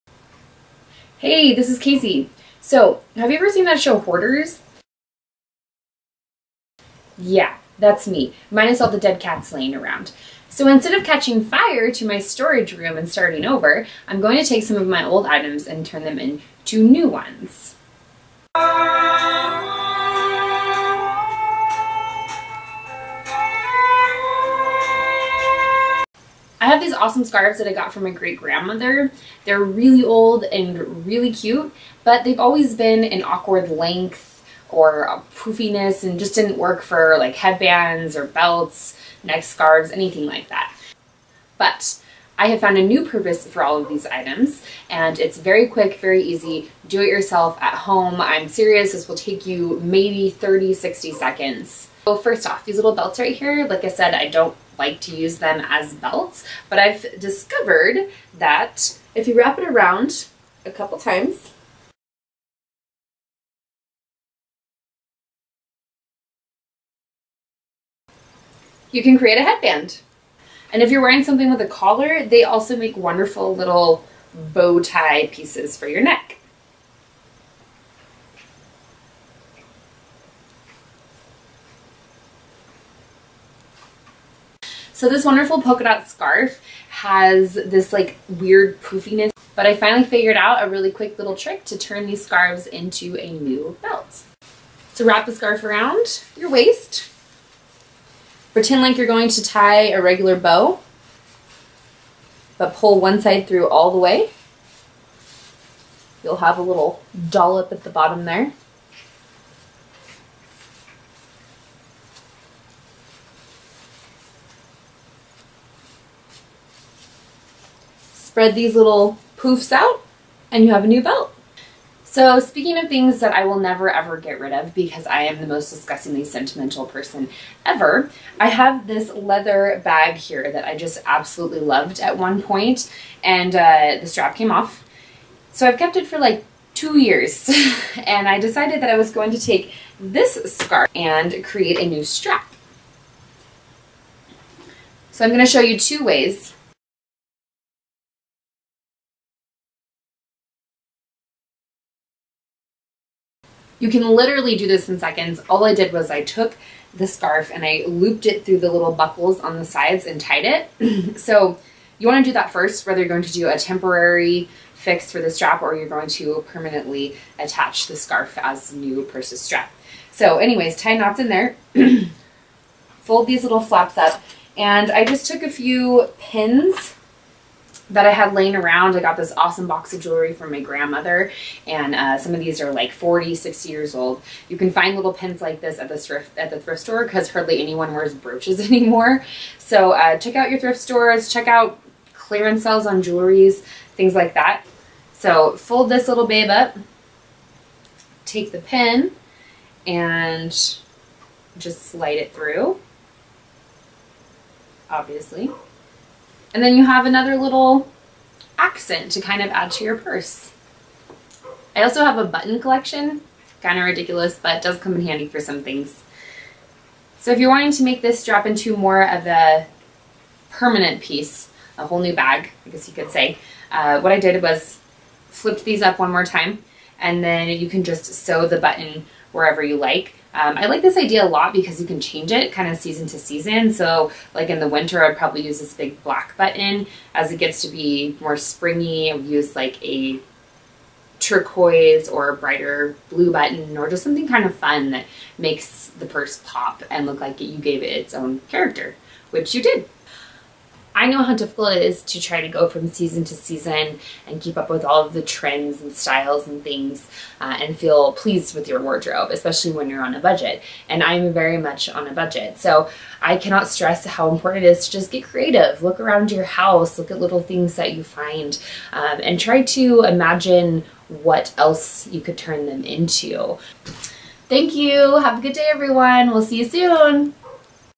DIY How To Make A New Purse From Old Scarves!
A few ways to turn old vintage scarves into new accessories!